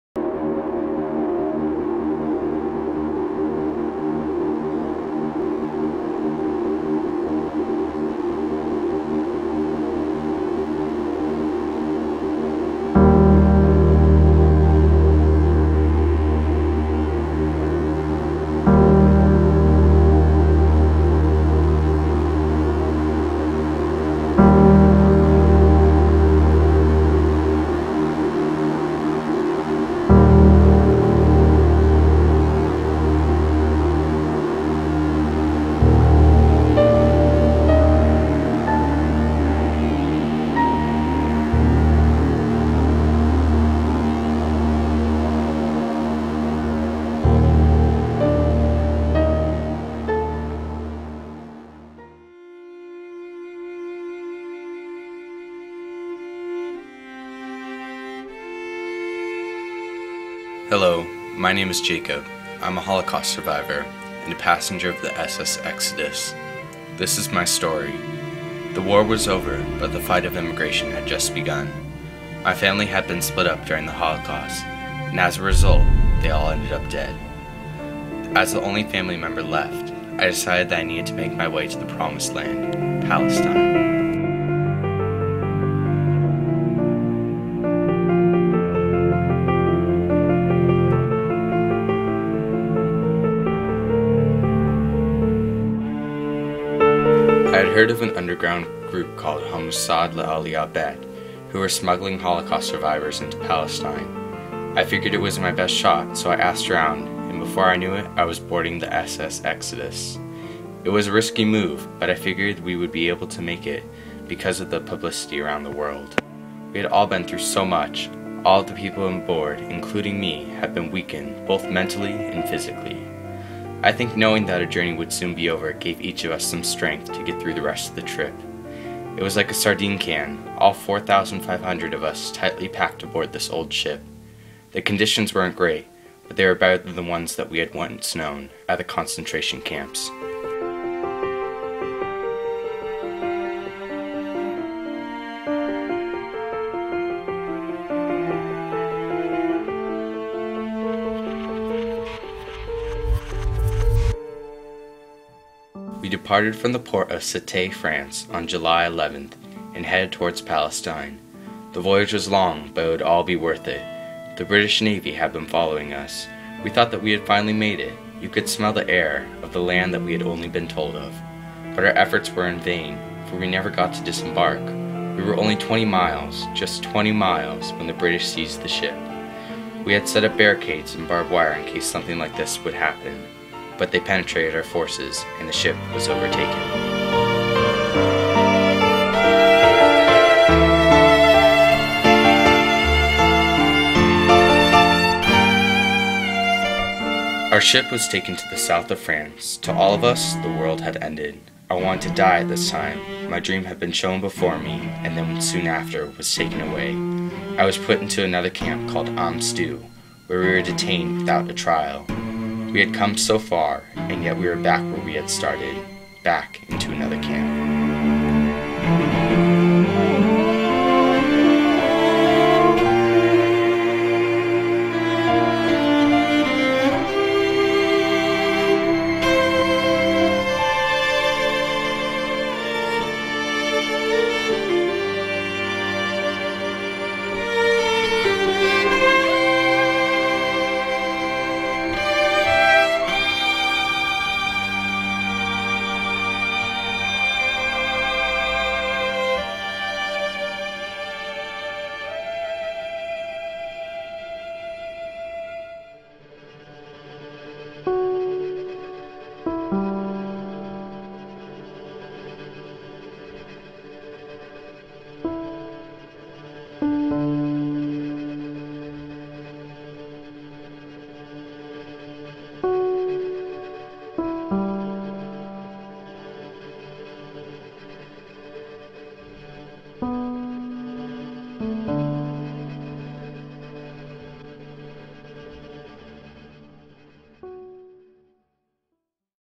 SS Exodus by Zach Robin
The Story of the SS Exodus. After the holocaust displaced Jews try to make their way to Palestine. They have to do this illegally because the British Mandate restricted emigration